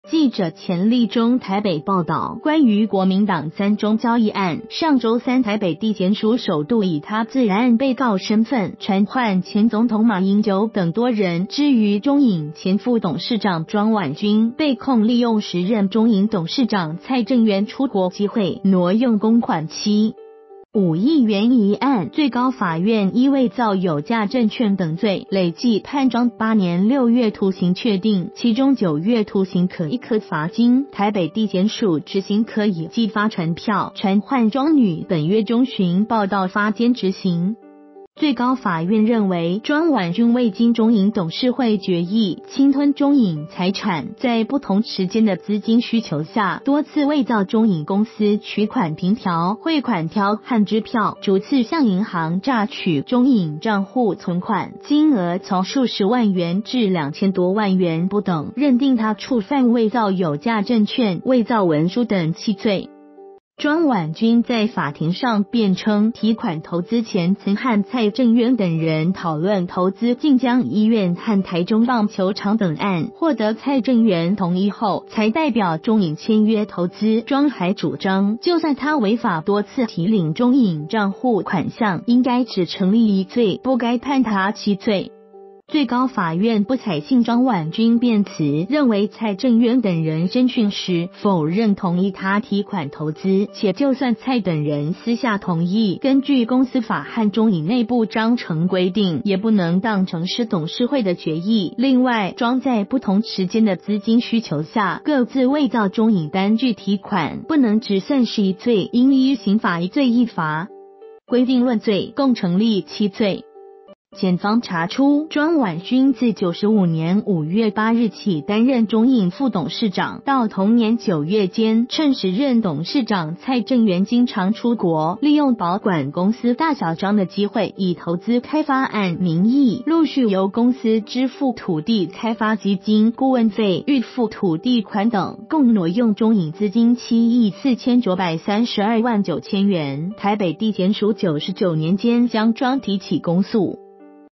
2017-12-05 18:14
〔記者錢利忠／台北報導〕關於國民黨三中交易案，上週三台北地檢署首度以他字案被告身分，傳喚前總統馬英九等多人；至於中影前副董事長莊婉均，被控利用時任中影董事長蔡正元出國機會，挪用公款7.5億元一案，最高法院依偽造有價證券等罪，累計判莊8年6月徒刑確定，其中9月徒刑可易科罰金；台北地檢署執行科已寄發傳票，傳喚莊女本月中旬報到發監執行。